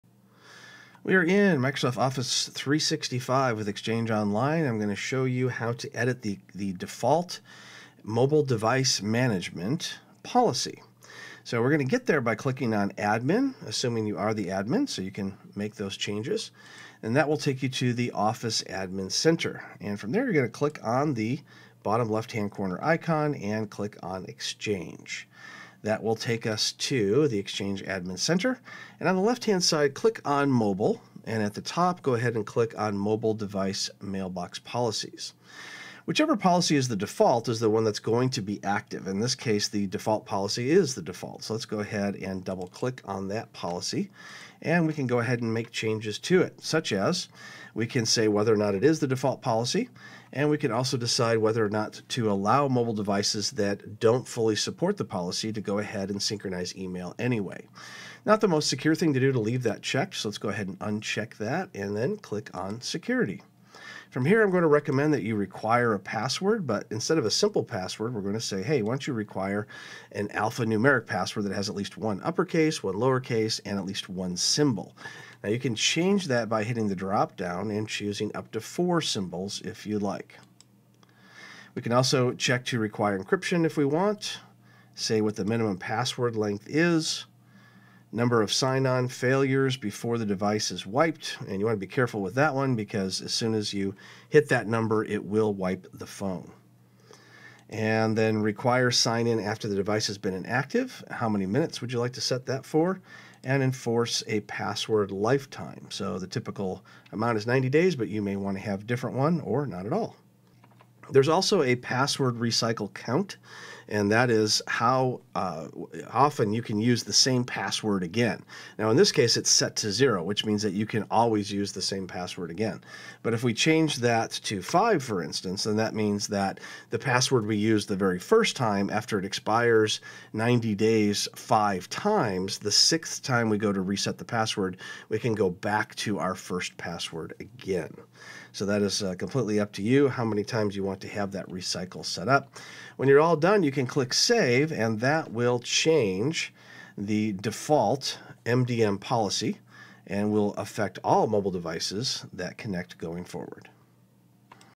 Microsoft 365 Tutorial How to edit the default MDM policy in Exchange Online
Professor Robert McMillen shows you how to edit the default MDM policy in Exchange Online. Microsoft 365 includes a mobile device management policy but it may not be setup as securely as you need it to be.